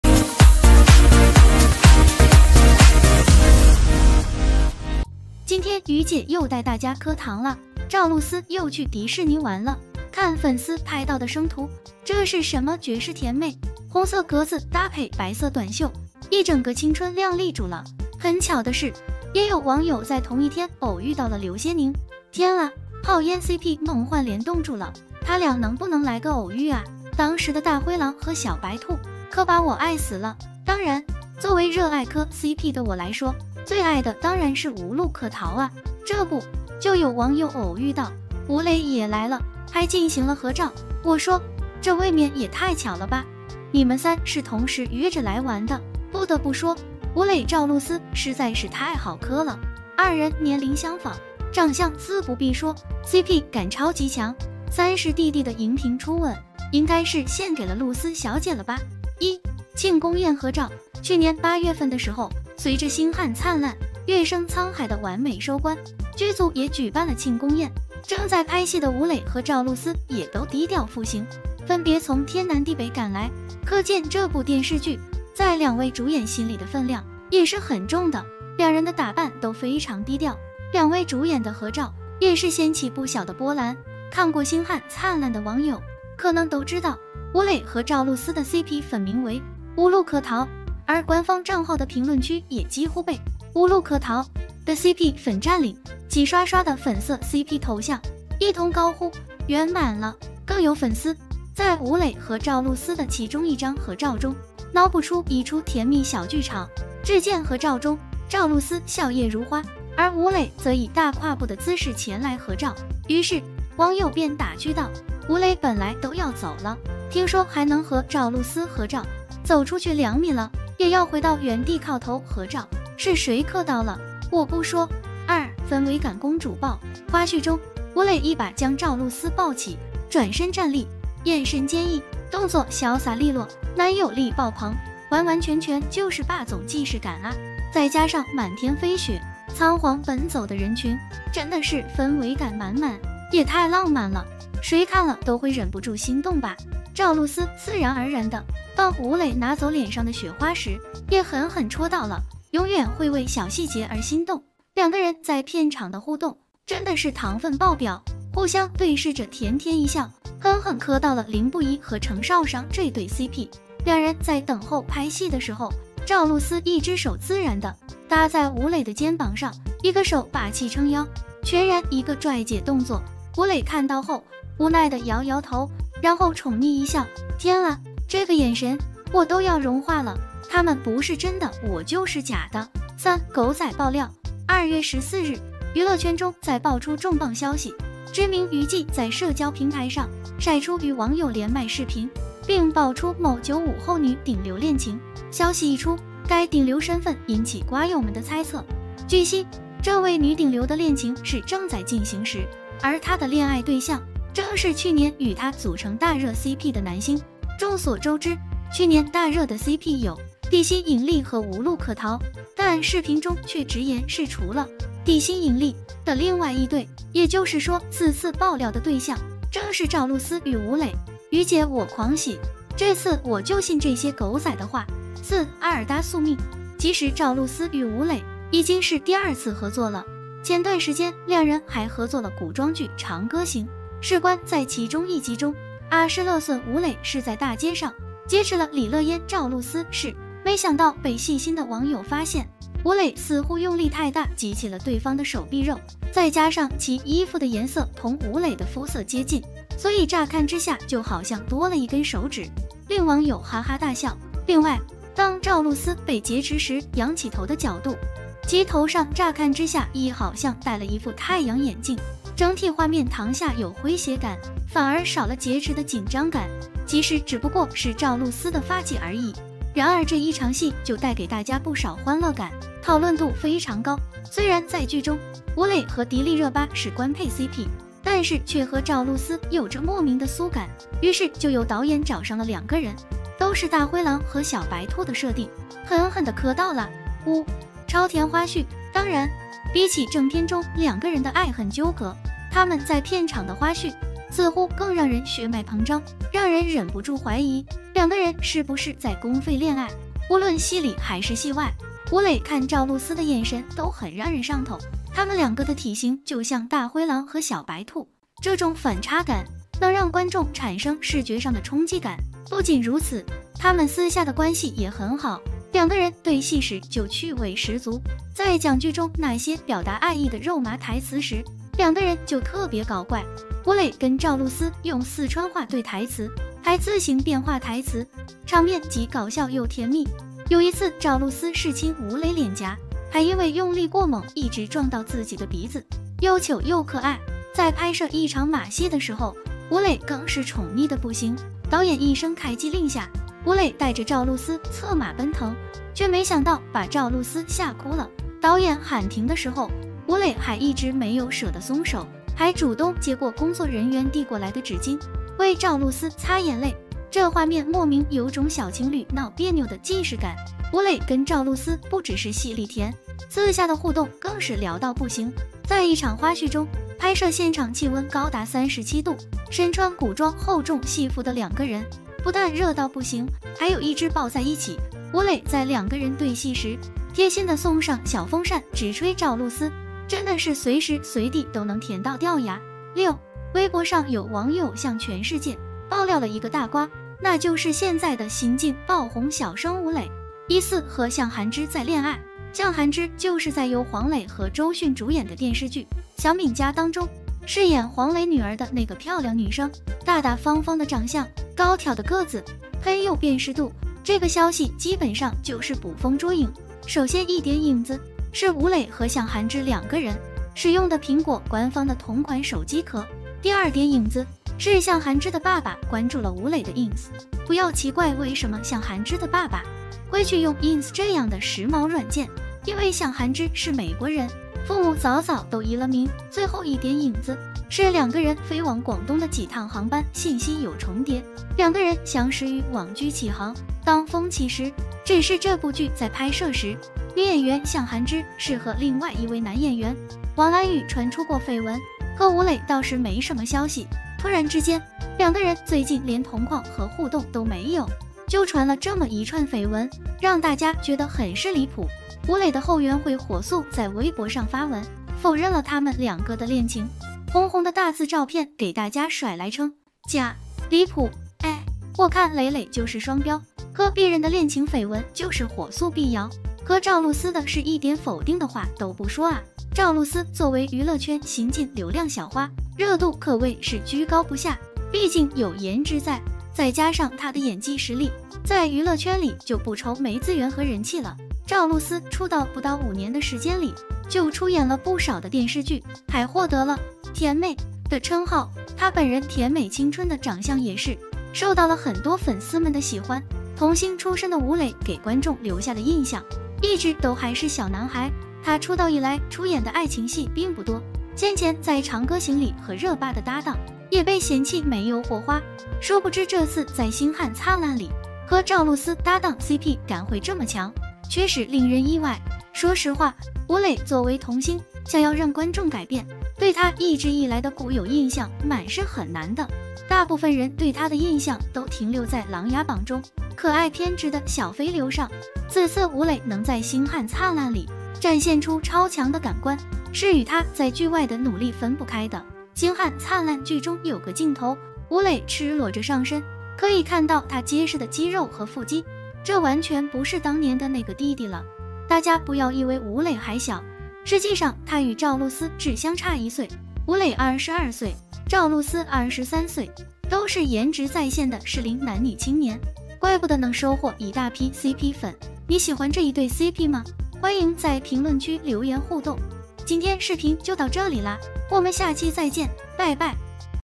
吳磊趙露思戀情石鎚！同逛迪士尼拍甜蜜合照，男方戴口罩打扮低調，吳露可逃果然是真的！吴磊
網友在迪士尼分別偶遇趙露思和劉宇寧！不知兩人會不會偶遇呢？同時，還有網友稱，也偶遇到吳磊了！還和路人進行了合影，雖然戴著口罩，但是不難看出，就是吳磊本人！這讓人不禁聯想到，難道是趙露思吳磊偷偷約會去了？吴磊 赵露思 cp #